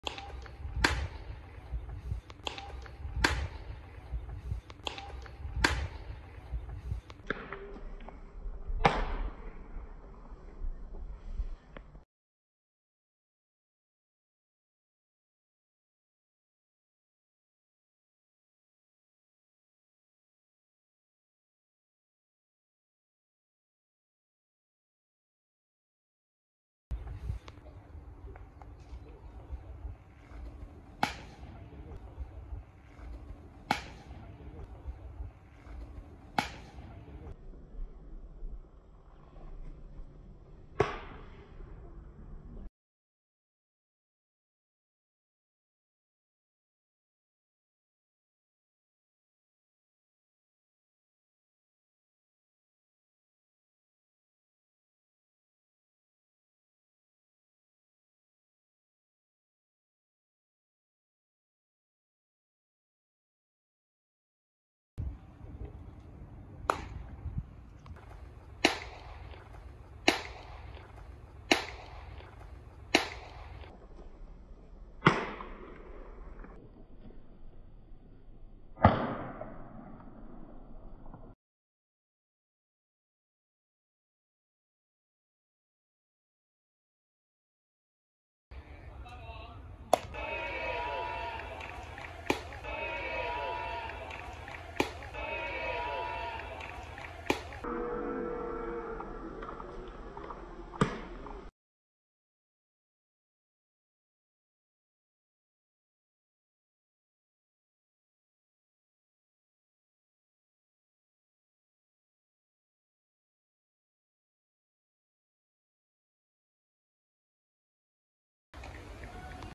ヤクルトスワローズ 跳ねる！跳ねる！ 今野龍太 走り幅跳び投法
最速153km/hの速球が武器
変化球はカットボール、フォーク、カーブを中心に投げ、稀にスライダーも投げる

ヤクルトスワローズ　山野太一　支配下目前か？　フォーム改造
youtu.be/ChDlZPYkCys